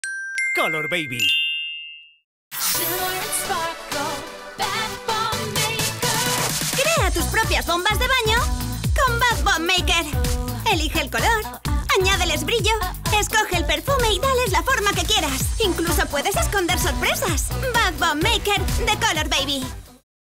Spot Máquina bombas de jabón Spa Shimmer ‘n Sparkle | COLORBABY
Es hora de tomar un baño super relajante con las bombas de baño Shimmer ‘n Sparkle 🛀
¡Las puedes hacer tú mism@! Mezcla varios colores, pon purpurina, ¡y hasta con aromas!